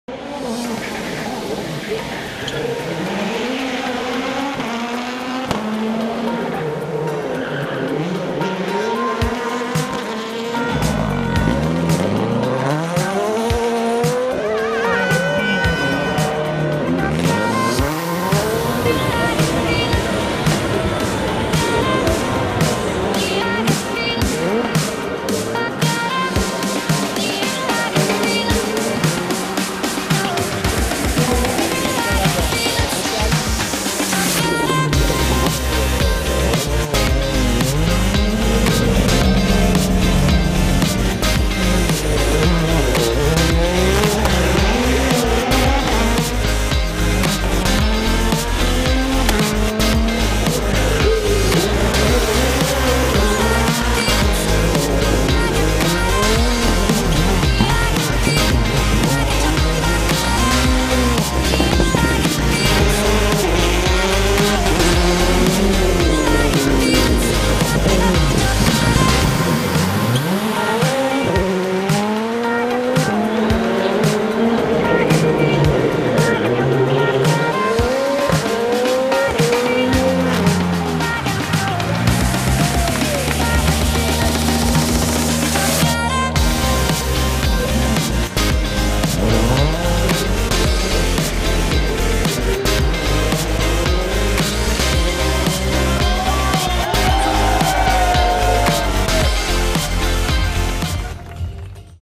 El musical del Ra·li Catalunya-Costa Daurada 2017
És tan sols un tast però serveix per gaudir de l'altra cara de la cita del motor més important del calendari internacional. Són els millors moments dels trams especials celebrats a Salou i les cerimònies d'inauguració i clausura del Passeig Jaume I.